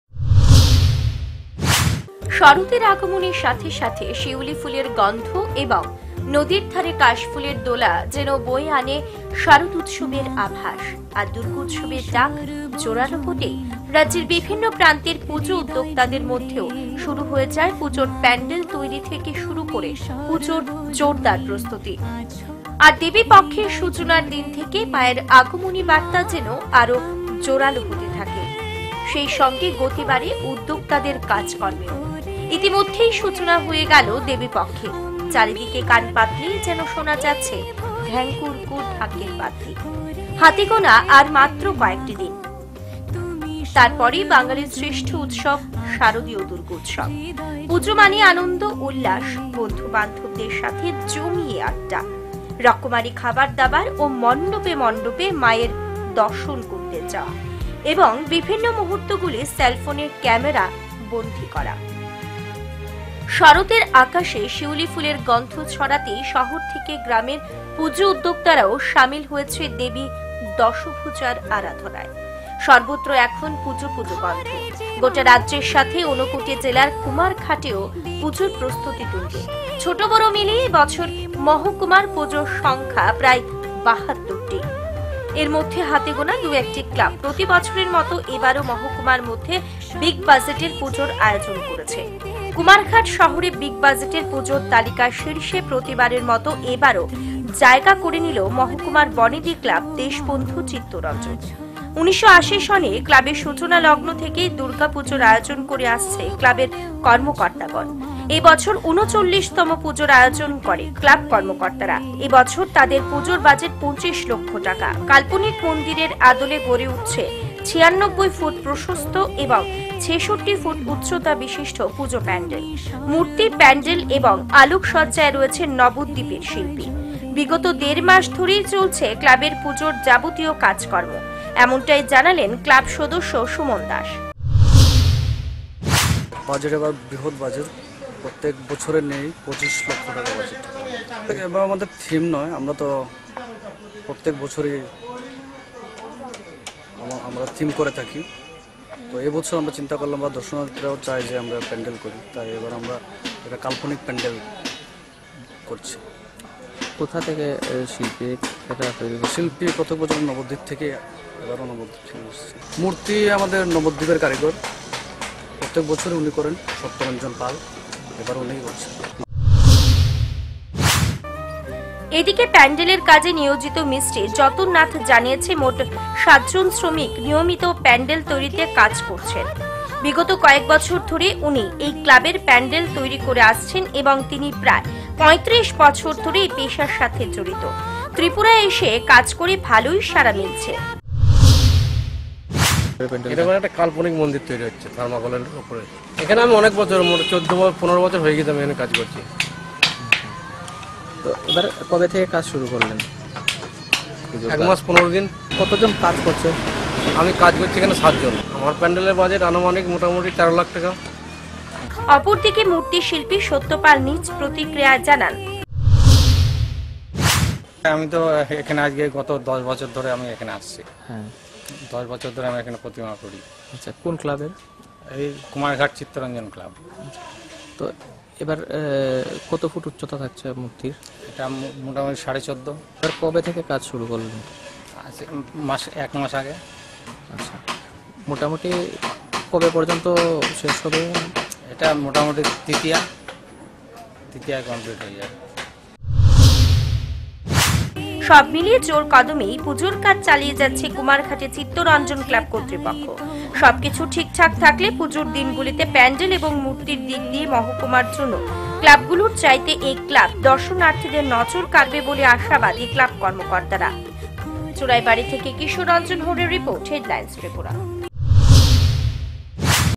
# রাজধানী আগরতলার পাশাপাশি মহকুমা সড়কগুলিতেও #
রাজধানী আগরতলার পাশাপাশি মহকুমা সড়কগুলিতেও বড় বাজেটের পুজোর প্রতিযোগিতায় মেতে উঠেছেন উদ্যোক্তারা। কুমারঘাটের দেশবন্ধু চিত্তরঞ্জন ক্লাব এবার ২৫ লক্ষ টাকা বাজেটের পুজো করে তাক লাগিয়ে দিতে চাইছেন গোটা ঊনকোটি জেলাকে। যদিও এই পুজোটি বরাবরই গোটা জেলার বড় বাজেটের পুজো।